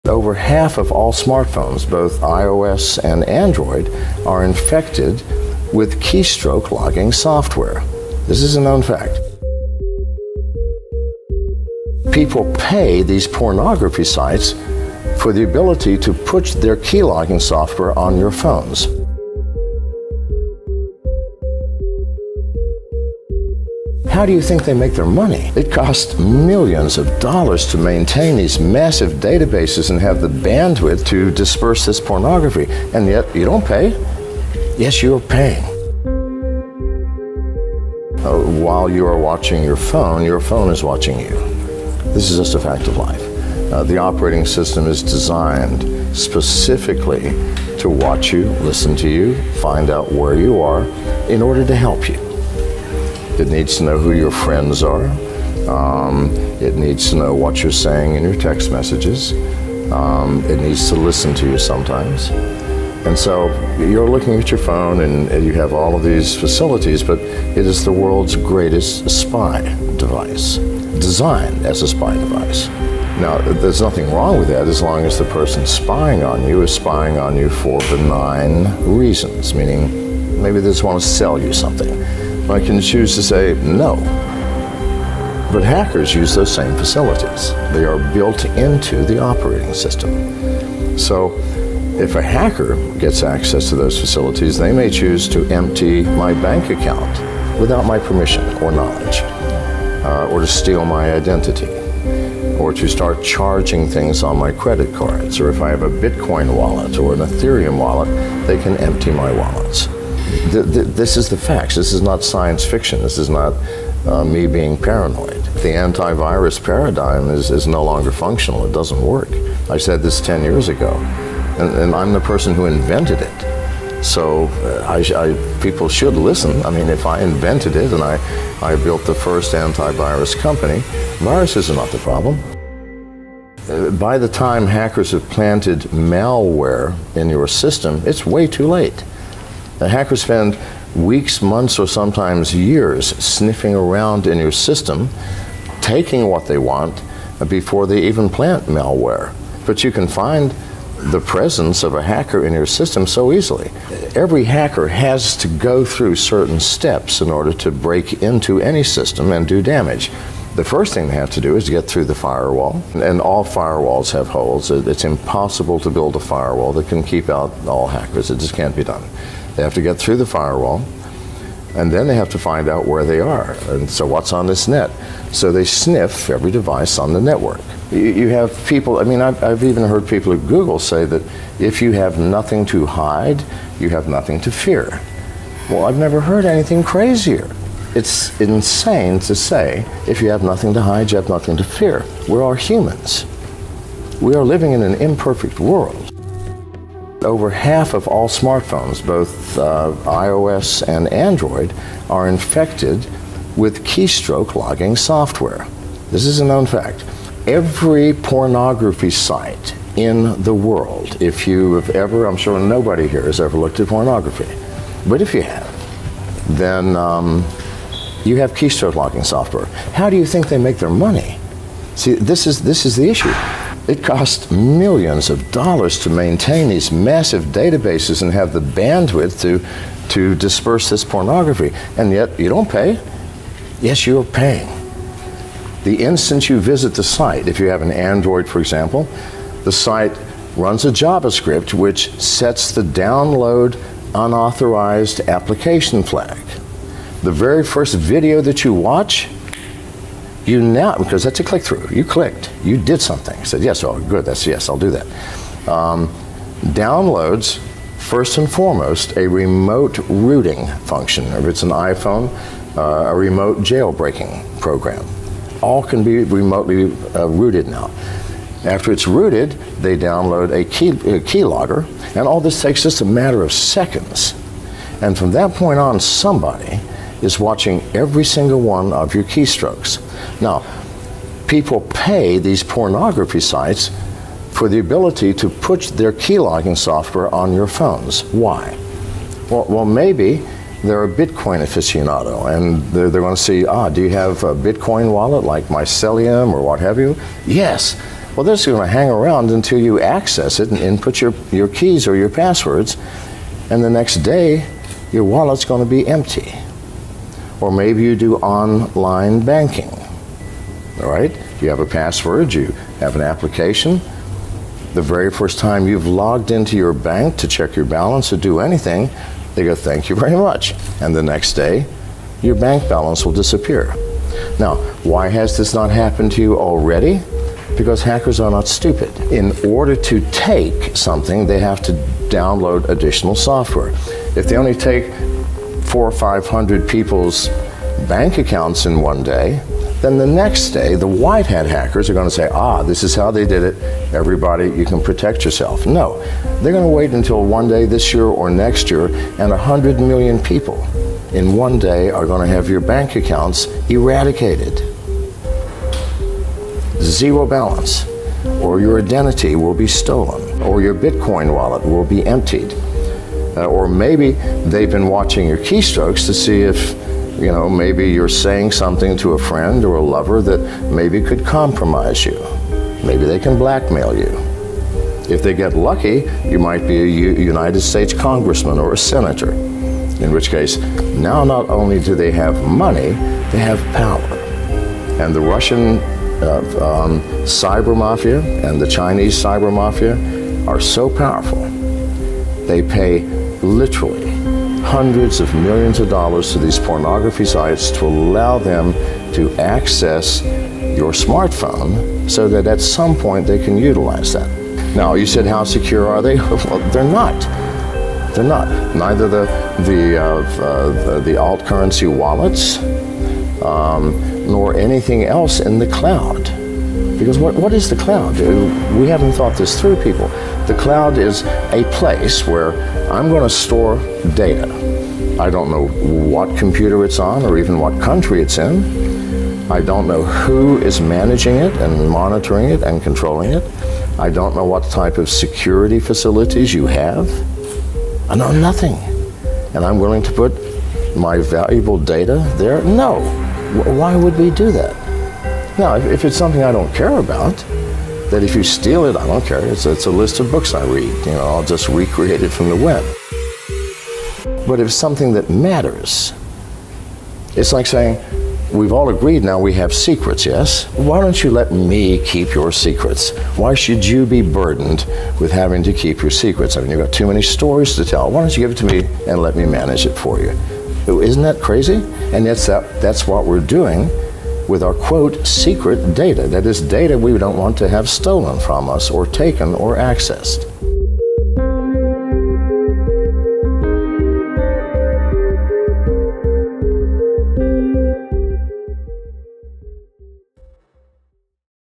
John McAfee: "You are immediately infected with KEYSTROKE LOGGING SOFTWARE"
"They can watch you and listen to you." - John McAfee

✅ SELF-HYPNOSIS AUDIO PROGRAMS (Reprogram Your Subconscious)

Don’t worry. We only recommend products we know and trust.